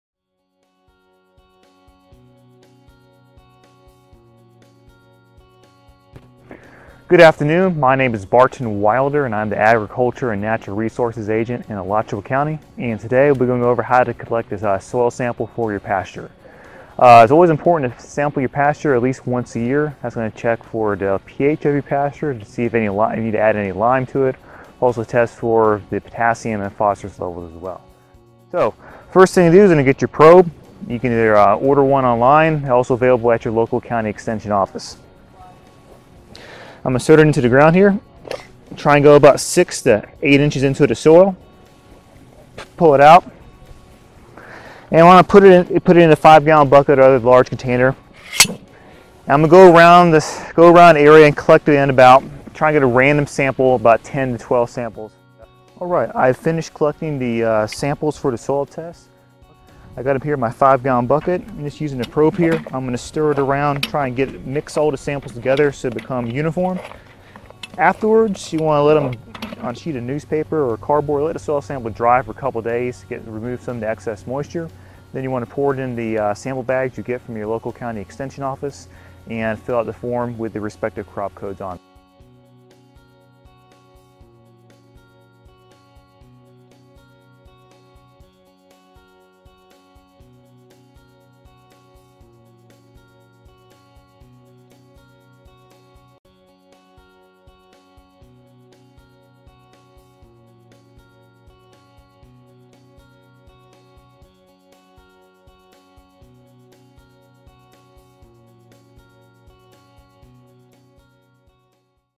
How to Take a Soil Test.mp4
This video explains the procedures for taking a soil test on your property.